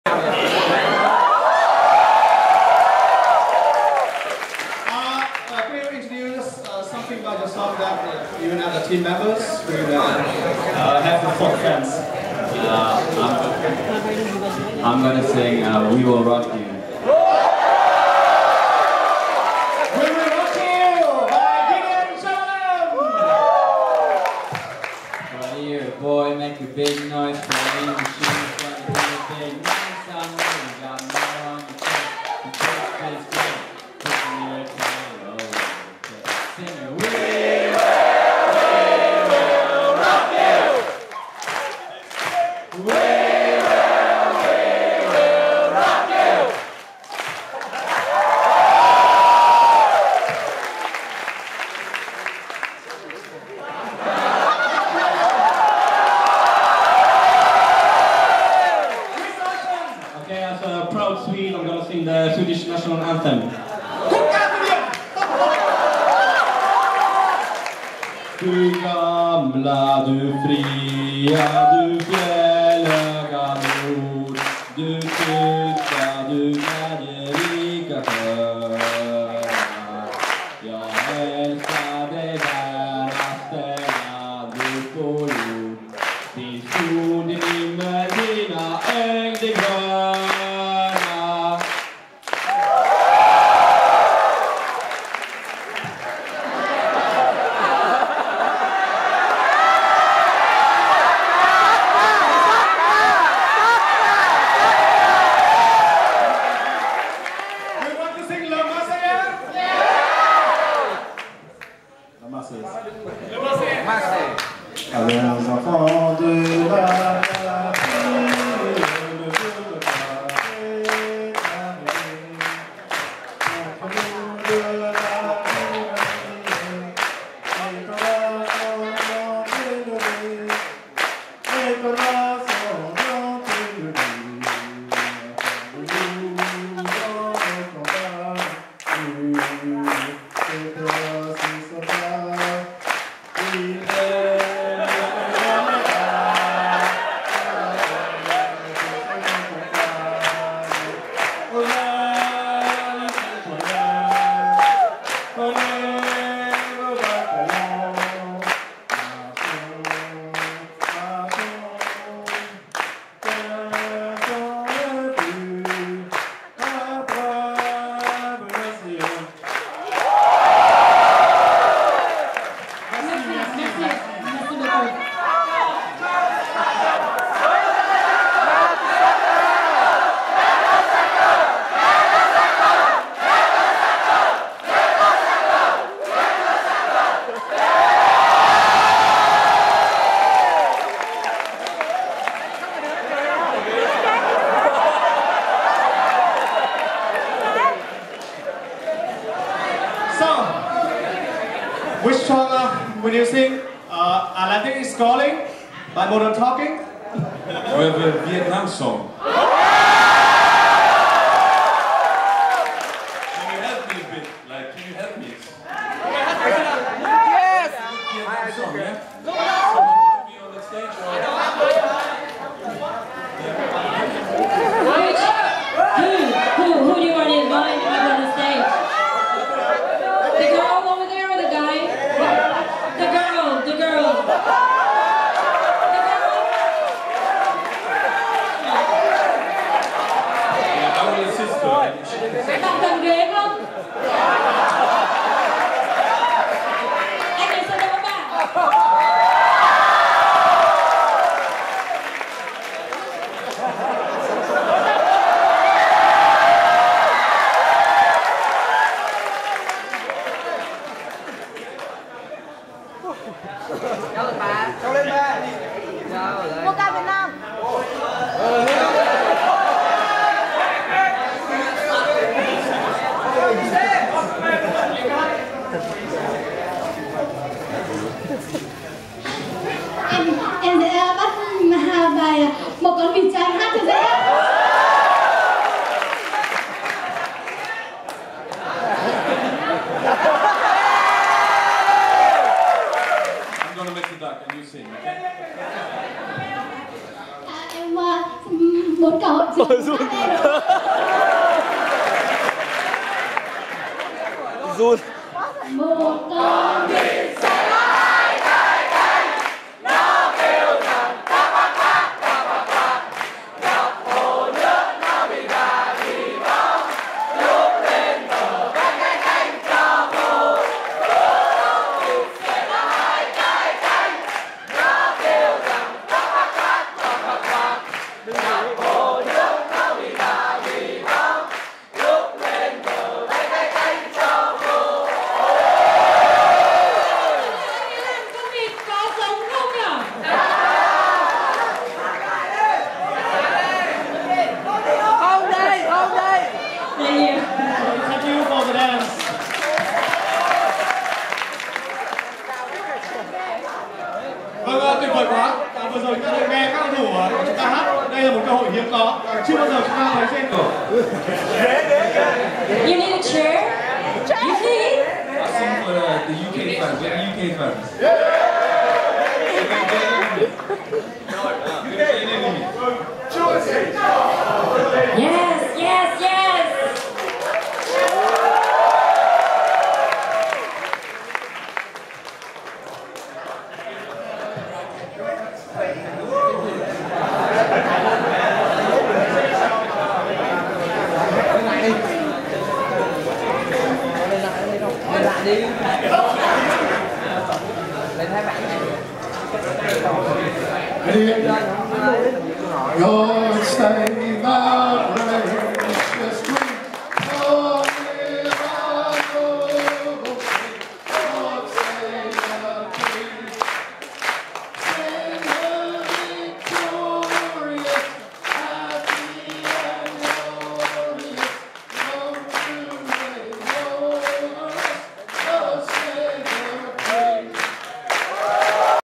The Gunners sing in Fan Party, Hanoi 2013.
Per Mertesacker, Bacary Sagna, Gedion Zelalem, Kris Olsson and Chuks Aneke joined in the fun at a Fan Party at the team's hotel in Hanoi on Tuesday, 16/7.